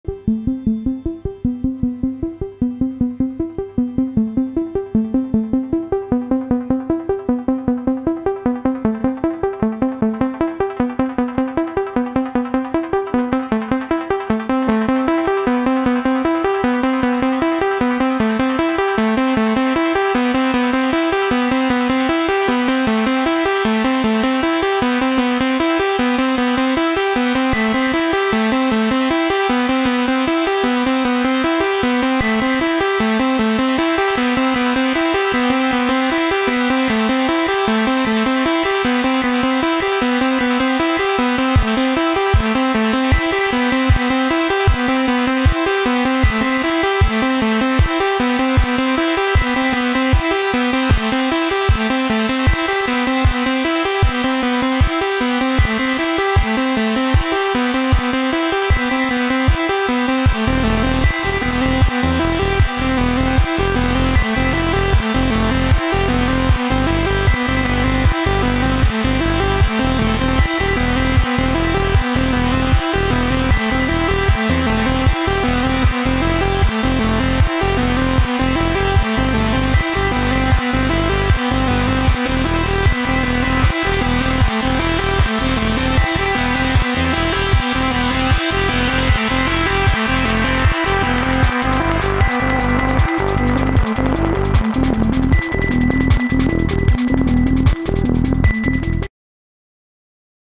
SACRIFICIO - Flashparty 2019 - Accelerated Demo
This is my remote entry for the Accelerated Demo category at the Flashparty 2019 demoparty.
Code by Felipe Alfonso (bitnenfer)